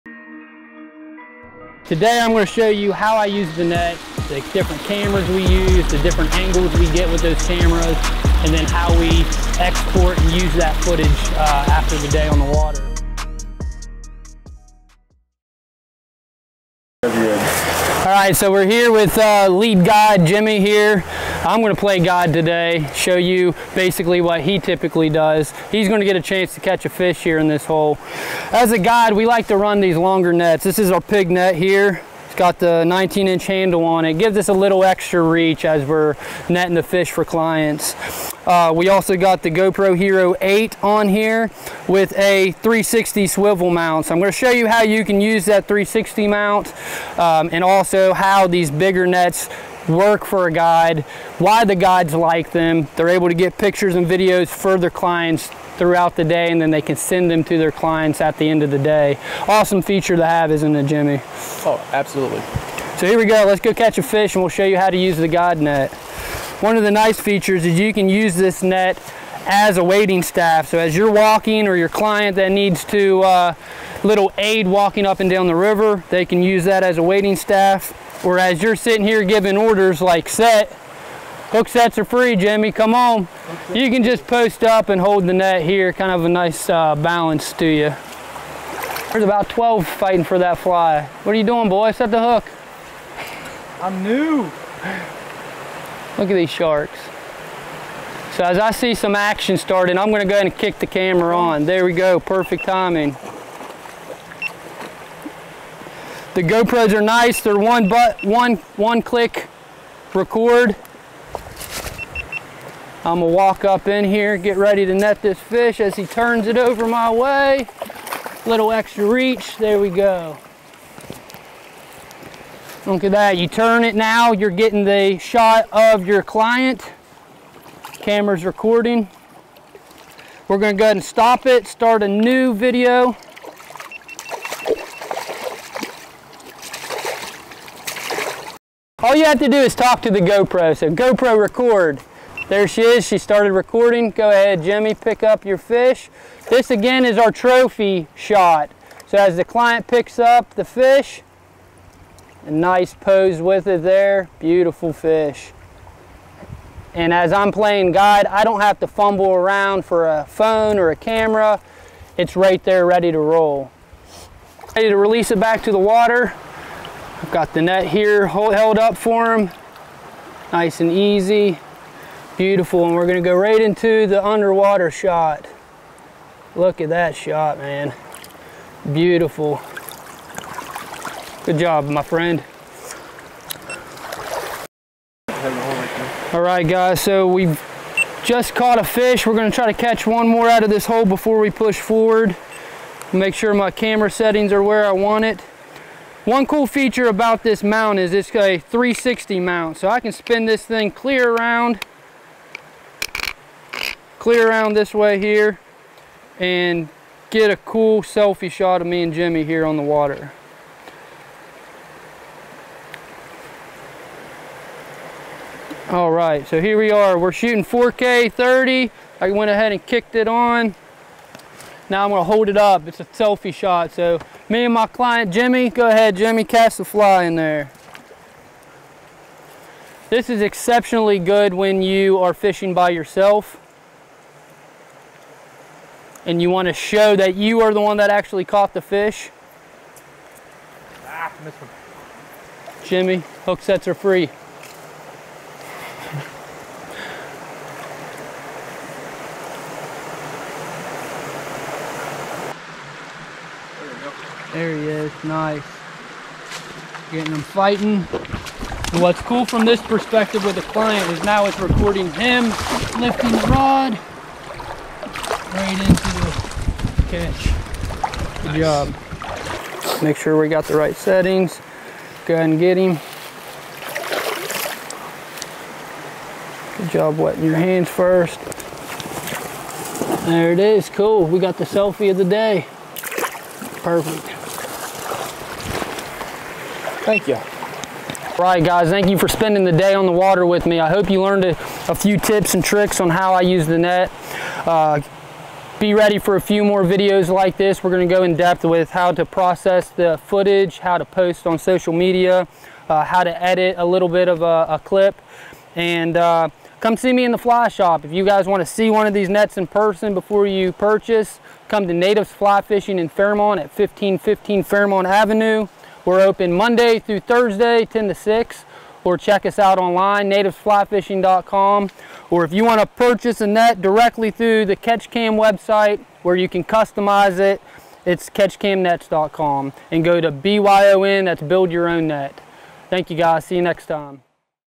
Catch Cam Nets How-To Series: Part 3 - The Ultimate Guide Net and Selfie
Part III of the series shows how we combine technology and our nets to make an easy and seamless way to take pictures and other content of your fish!